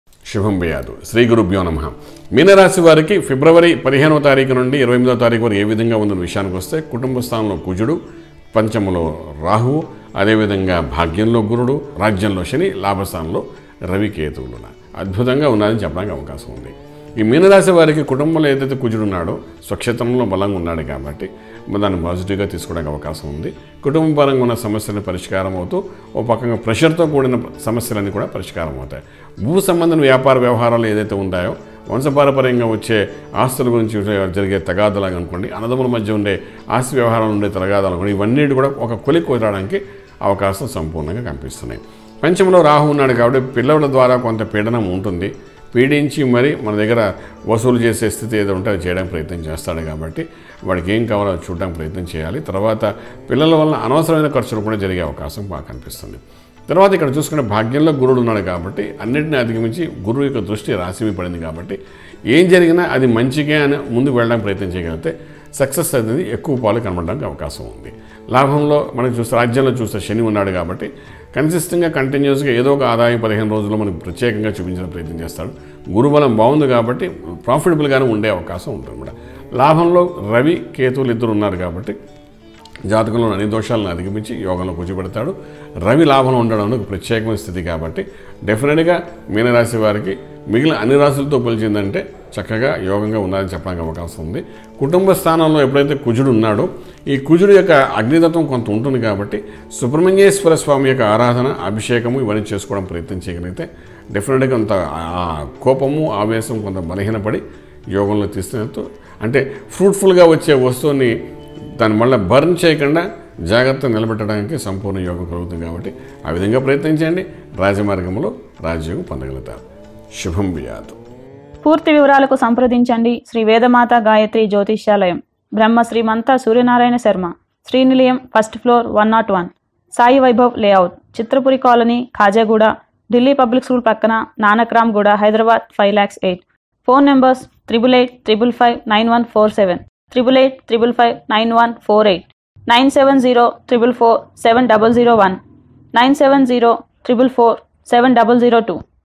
Meena Rashi phalalu | february | Pisces Horoscope | telugu astrology | Rasi phalalu 2019
మీన రాశి  వారికి ఫిబ్రవరి 15 నుండి
లాభంలో రవి రాకతో   ఆర్థిక అభివృద్ధి